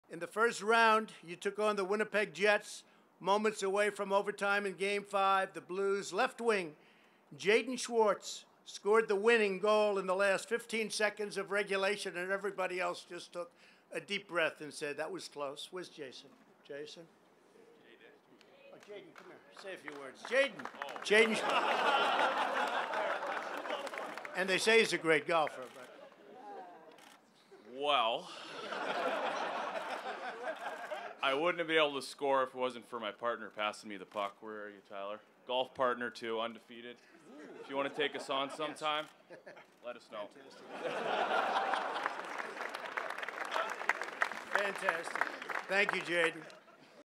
Donald Trump vs. Jaden Schwartz
Donald Trump and Jaden Schwartz share a small verbal exchange at the White House during the St. Louis Blues Stanley Cup Championship Ceremony.